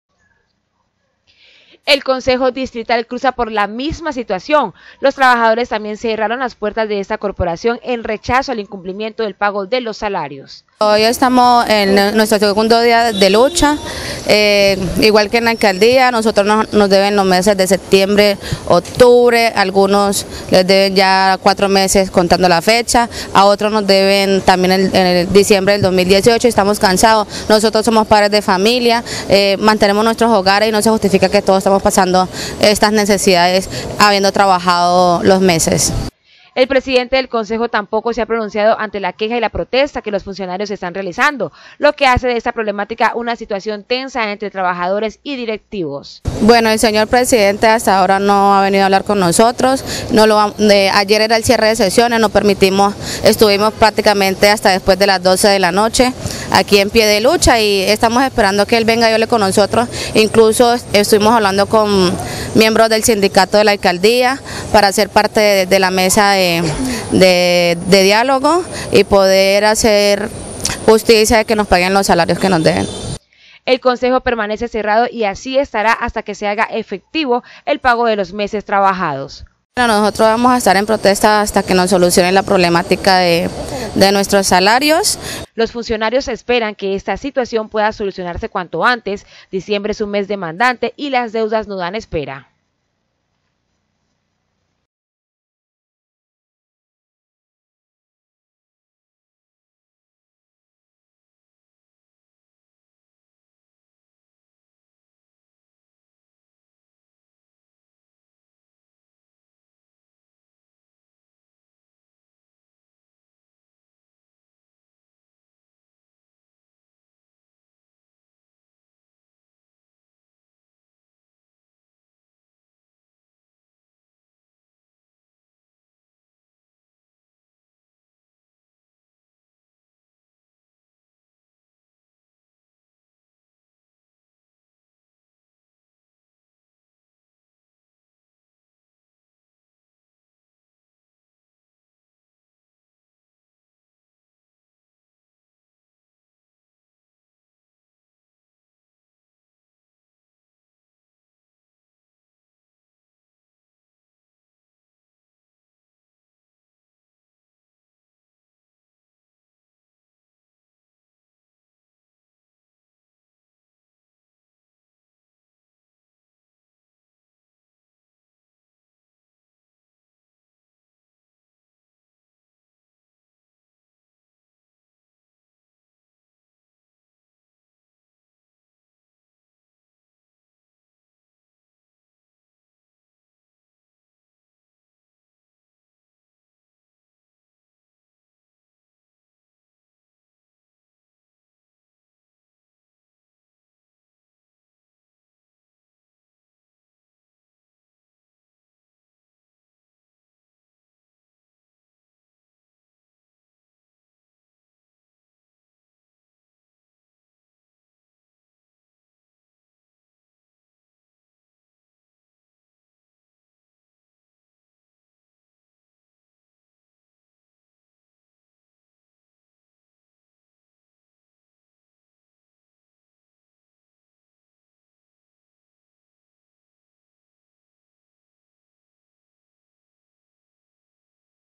SIN PAGO CONCEJO
Funcionarios del Concejo Distrital se unieron a la jornada de protesta porque a ellos tampoco se les ha cancelado el salario desde septiembre.